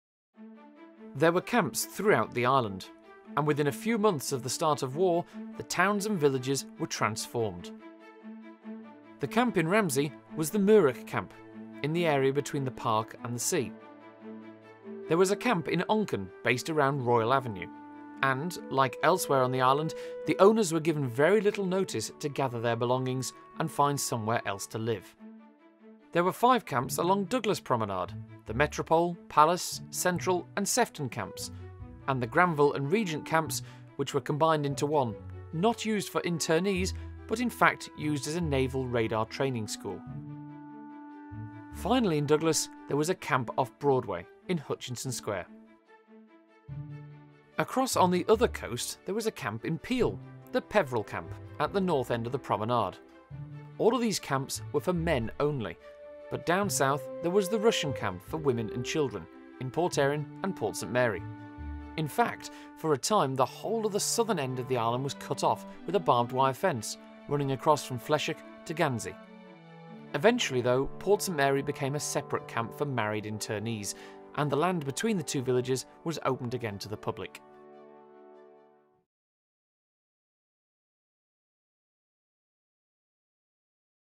Overview of WW2 Internment Camps in the Isle of Man
A short overview of the location and role of the internment camps in the Isle of Man dring the Second Wolrd War.
From the first opened at the Mooargh in Ramsey, via the many in Douglas and Onchan, and the special Peveril Camp for British Fascists in Peel, to the women and families' camp in Rushen.

This is one of a series of films investigating the Isle of Man during the Second World War.

More resources on the Isle of Man during WWII are available

This film was made in 2014 by Culture Vannin.
Presented by Alex Brindley. Written & Produced by Charles Guard. Music by Charles Guard.